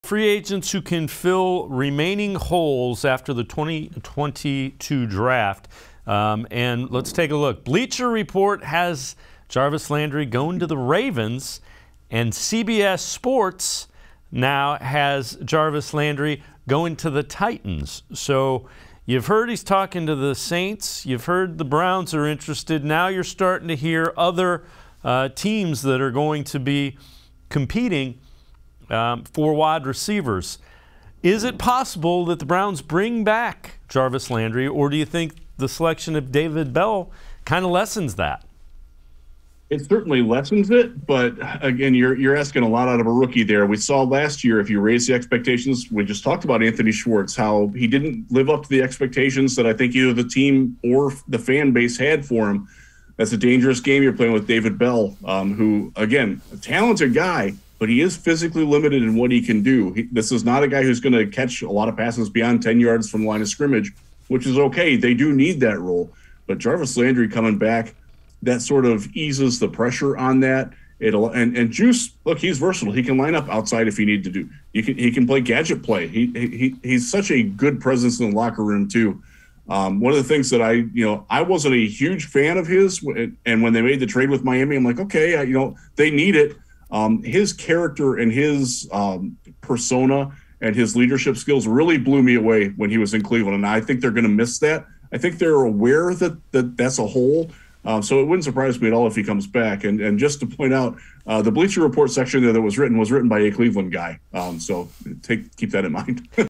Could Jarvis Landry Be Going to the Ravens? - Sports4CLE, 5/2/22
Dave Bacon and Jeff Risdon discuss former Browns wide receiver Jarvis Landry and where he could wind up this offseason.